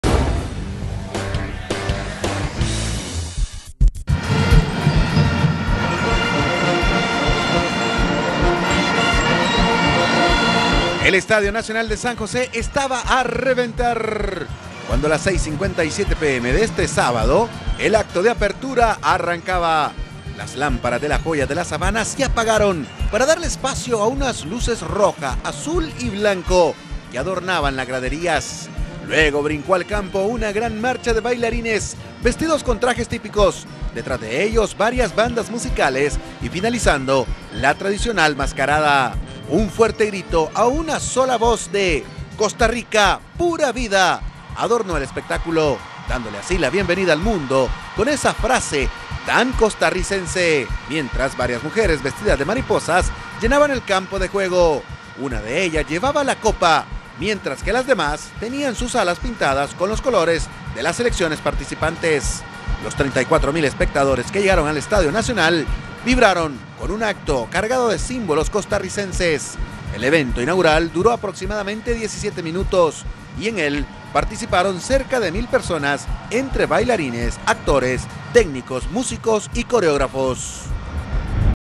TVC Cinco Deportivo- Inaugurado Mundial Sub 17 femenino en Costa Rica
Televicentro TVC tvcplay TelevicnetroHN Centro América se visto de gala, porque anoche se dieron los actos y arrancó el Mundial Sub 17 femenino en la ciudad de San José Costa Rica, convirtiéndose en la primera nación del istmo es ser sede de una copa Mundial de la FIFA.